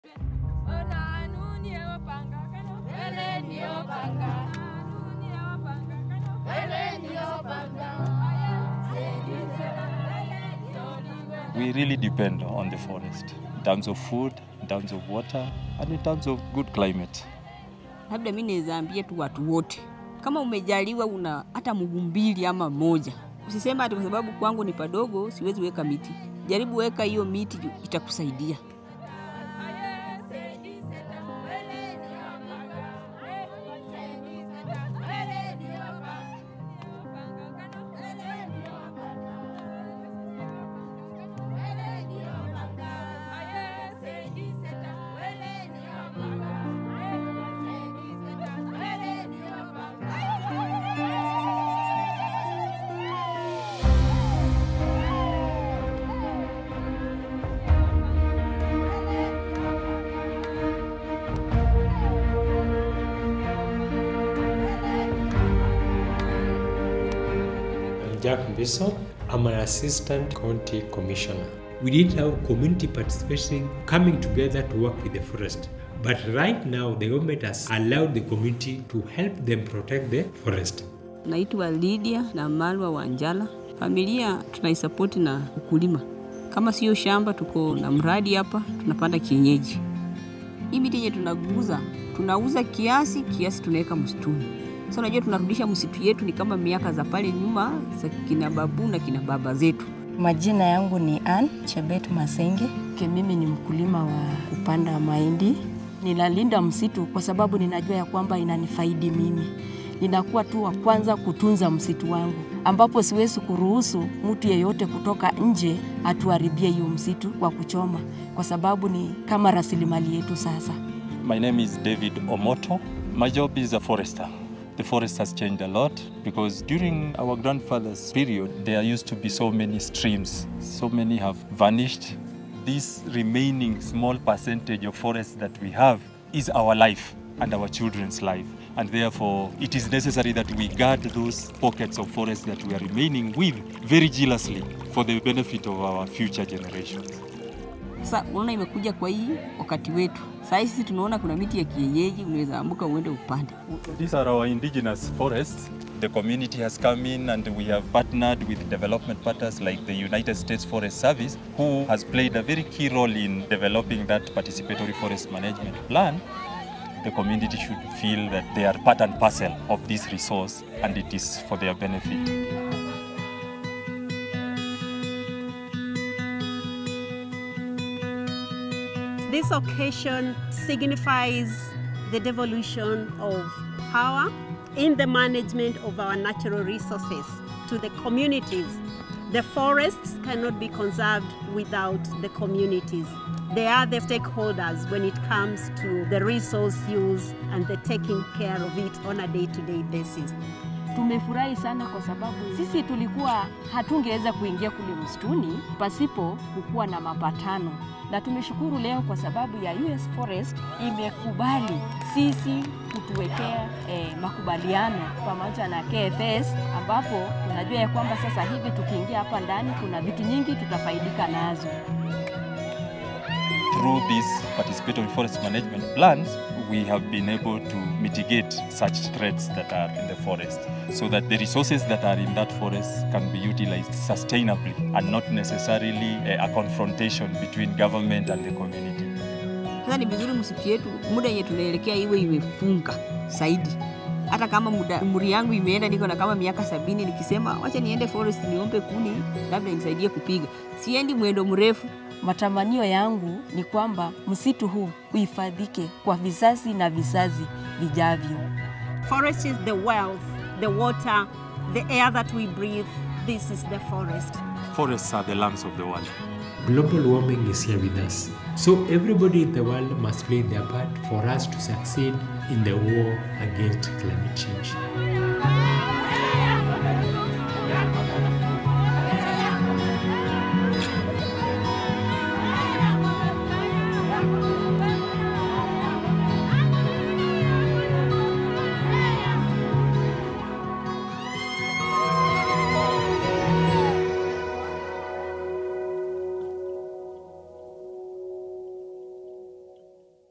Partner Video: Forest Service Mt Elgon Forest Participatory Management Plan 2024
Forests are our future! 🌳🌍

In the stunning Mt.Elgon area of western Kenya, farmers, foresters, and government leaders are coming together to protect and restore their forests for future generations. With support from  Kenya Forest Service,  USAID Kenya and the  U.S. Forest Service, they’ve developed participatory forest management plans that guide the stewardship of these vital watersheds.